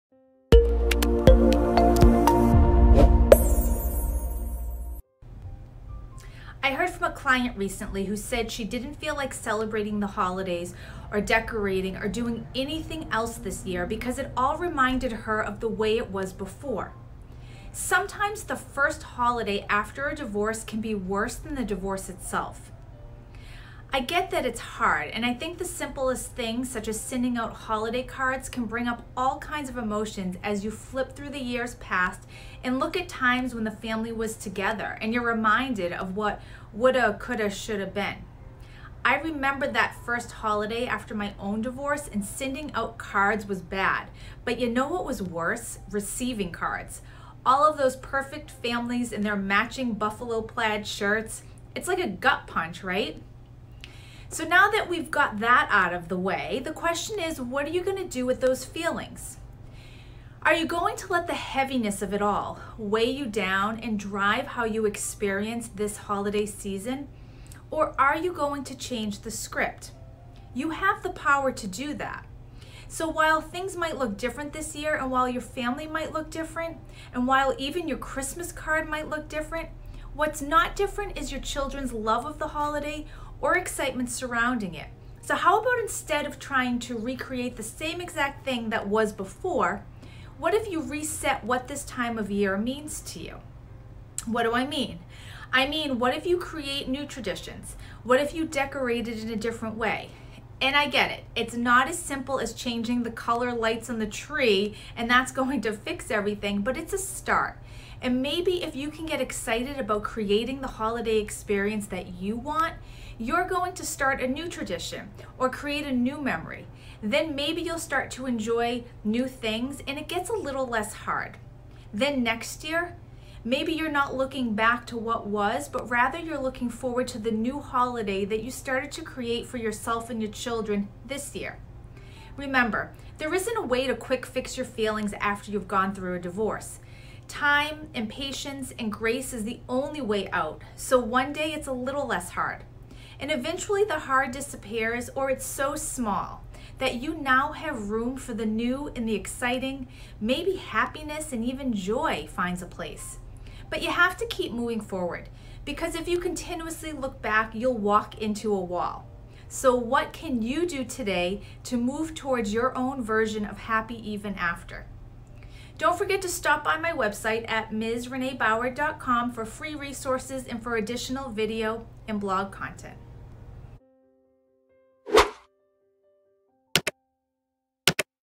Holidays After Divorce | Healing From Divorce | Renee Bauer
Holidays After Divorce: Holidays are usually happy occasion but it can also intensify feelings of sadness, loss, and failure, especially who are healing from divorce. For newly separated and divorced people, the holidays can really emphasize how much their lives have changed. Don’t worry, today Renee Bauer will tell you how you can overcome this.